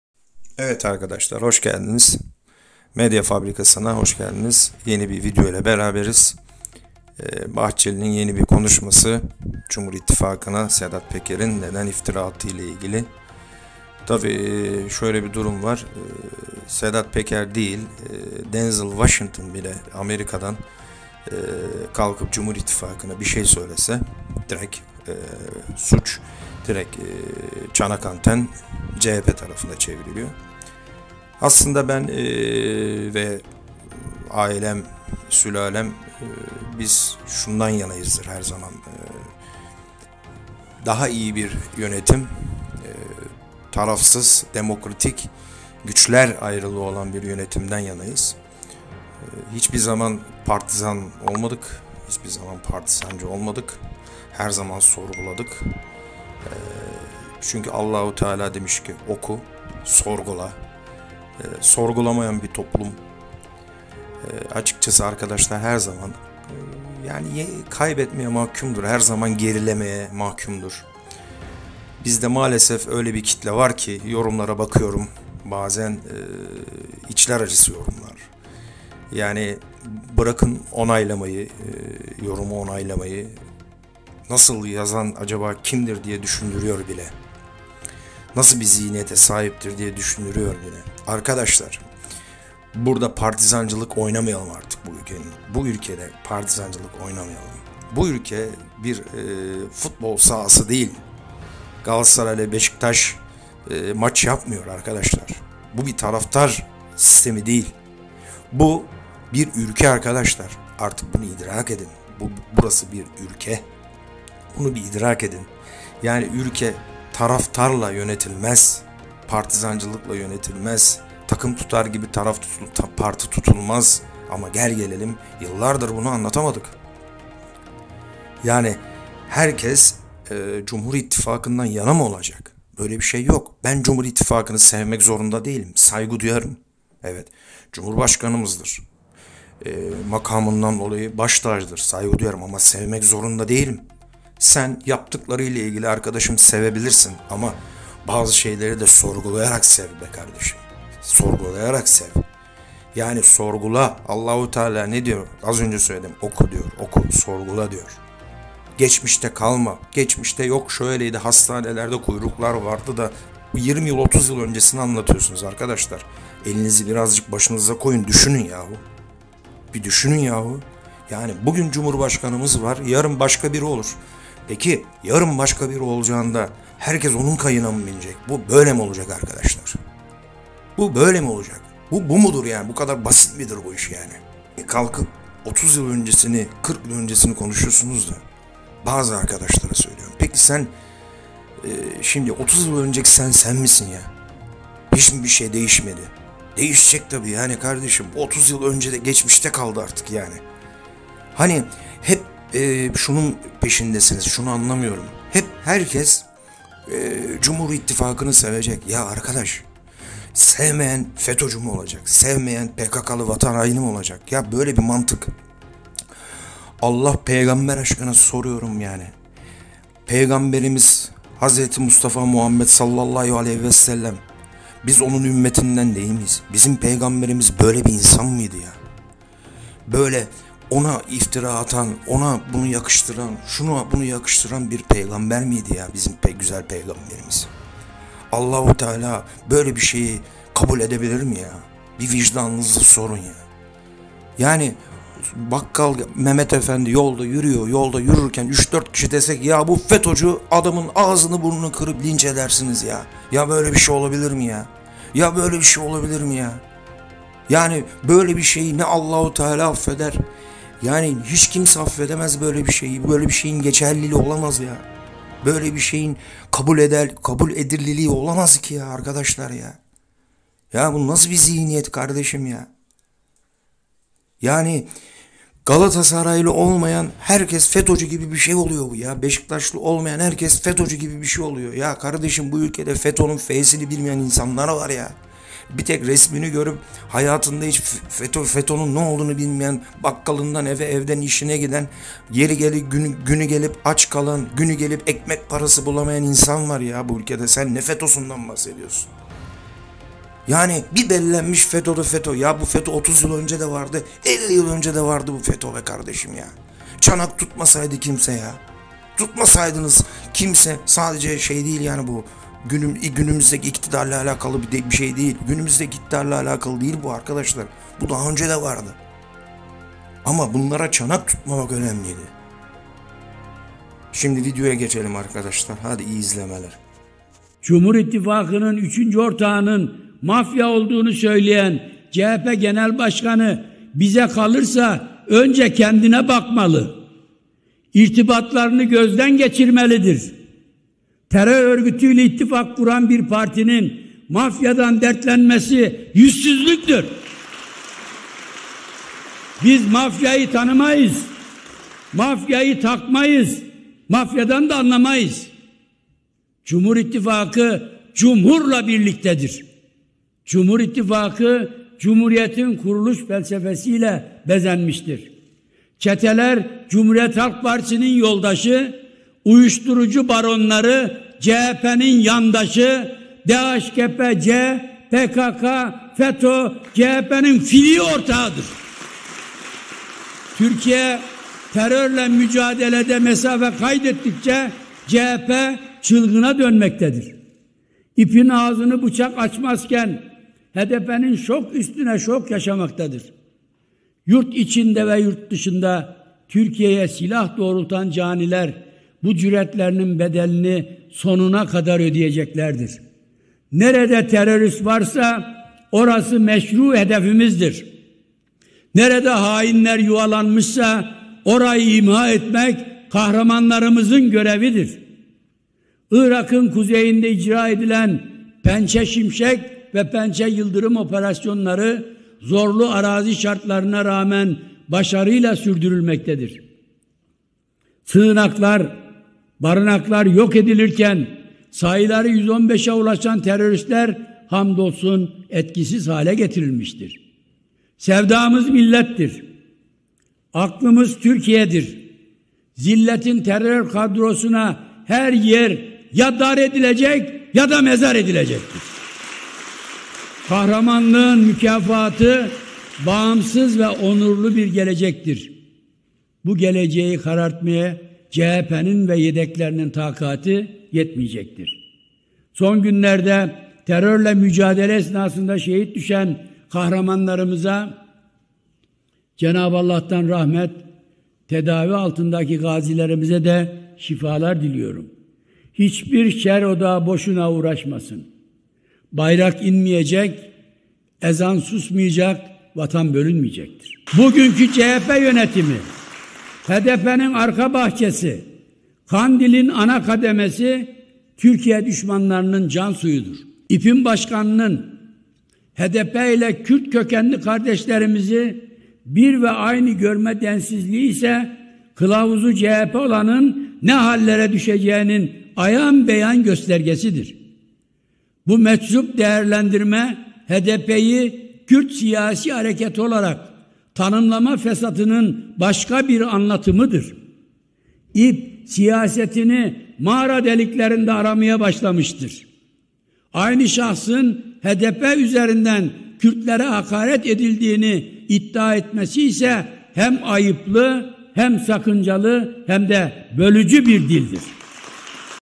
Bahçeli "Sedat peker'in kılıçdaroğlu ile bağlantısı vardır" mutlaka izleyin.
bahçeli sedatpeker kılıçdaroğlu sondakikahaberleri bahçeli "sedat peker'in kılıçdaroğlu ile bağlantısı vardır" mutlaka izleyin.